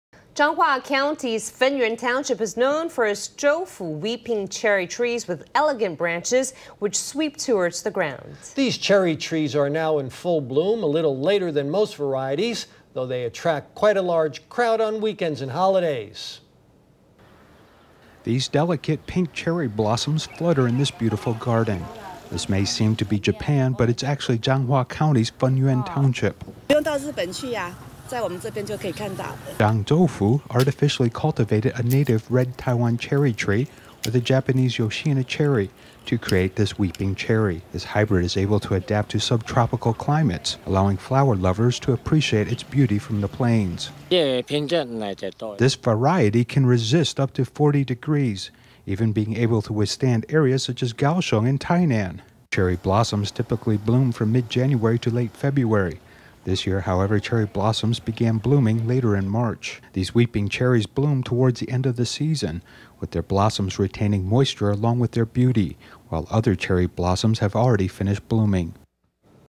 Changhua County’s Chou Fu weeping cherry trees in full bloom
Changhua County’s Fenyuan Township is known for its Chou Fu weeping cherry trees with elegant branches which sweep towards the ground. These cherry trees are now in full bloom, a little later than most varieties, though they attract quite a large crowd on weekends and holidays. These delicate pink cherry blossoms flutter in this beautiful garden. This may seem to be Japan but it’s actually Changhua County’s Fenyuan Township.Chang Chou-fu artificially cultivated a native red Taiwan cherry with a Japanese Yoshino cherry to create this weeping cherry. This hybrid is able to adapt to subtropical climates, allowing flower lovers to appreciate its beauty from the plains.Chang Chou-fuCherry Hybrid CreatorThis variety can resist heat up to forty degrees even being able to withstand areas such as Kaohsiung and Tainan.Cherry blossoms typically bloom from mid-January to late February. This year, however, cherry blossoms began blooming later in March.These weeping cherries bloom towards the end of the season with their blossoms retaining moisture along with their beauty while others cherry blossoms have already finished blooming.